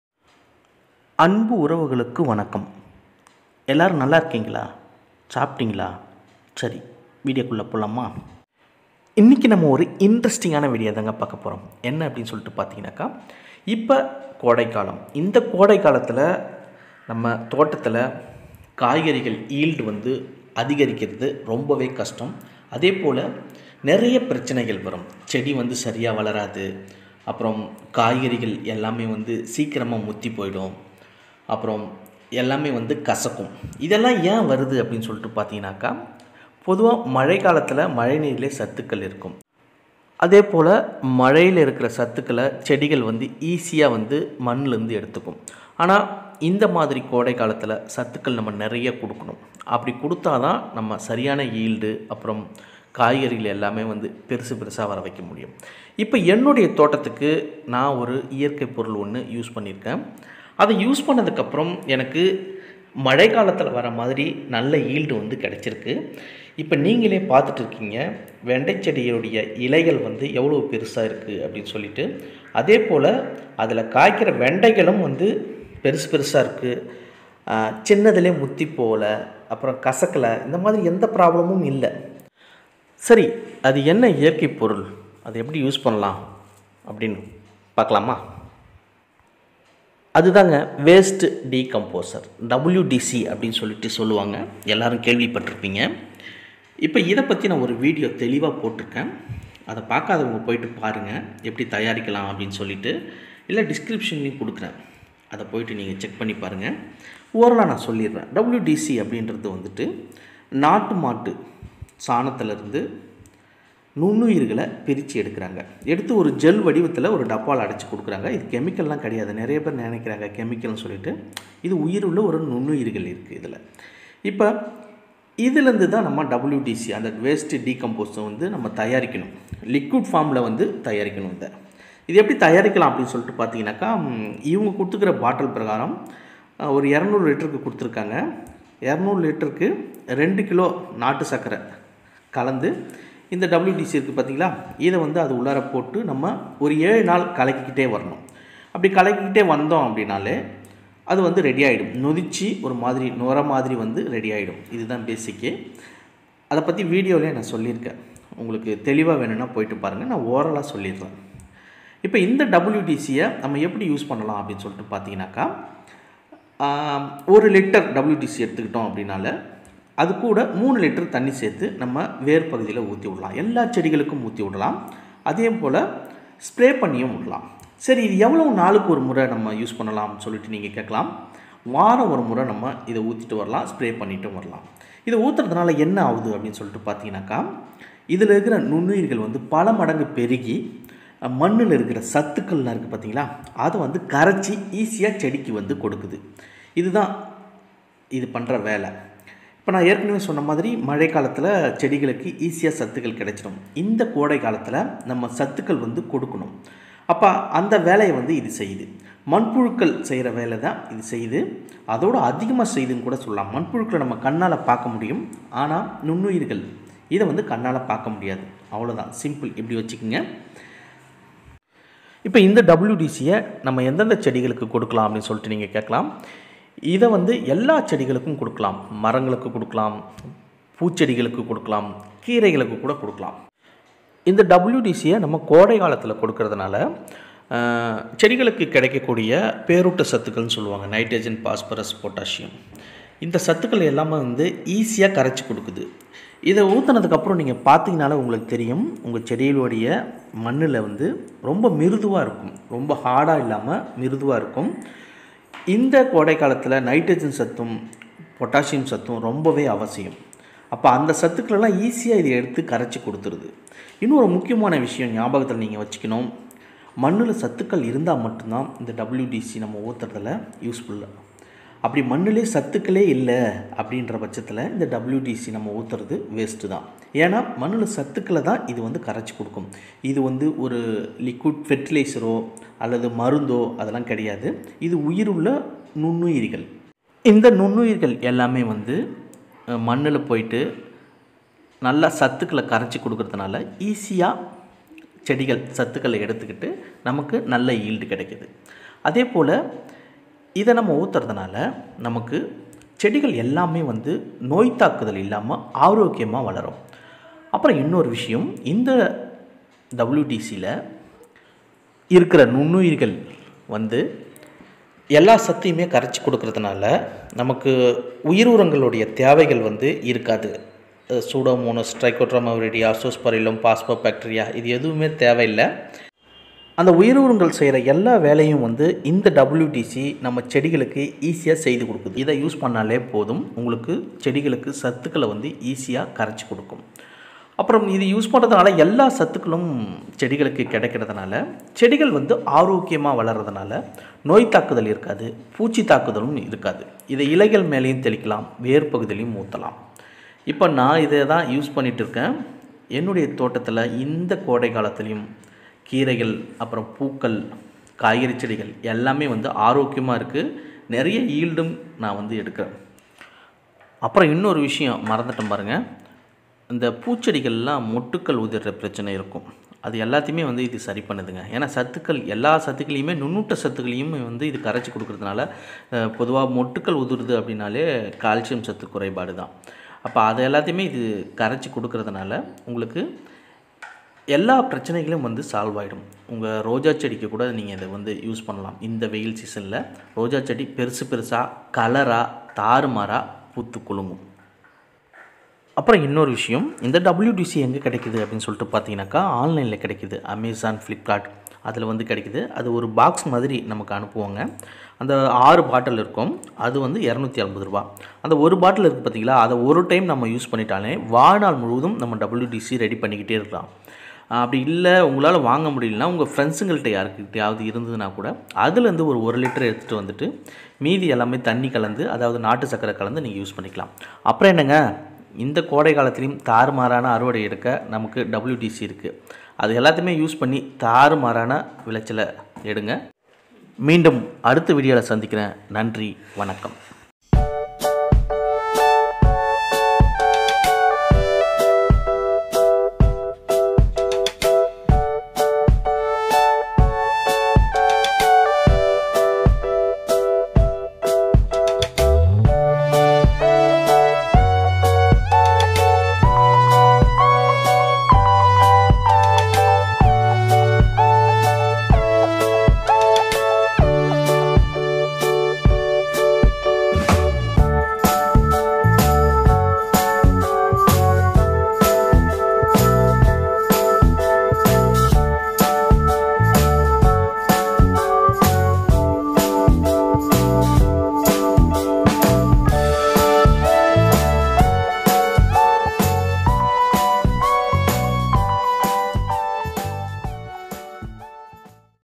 How to Use WDC | What are its Uses | What is the Result After using WDC | Waste DeComposer
In this video you can learn in detail how to use WDC to get more harvest in terrace garden in summer

WDC தயாரிப்பது எப்படி? How to Prepare WDC ??...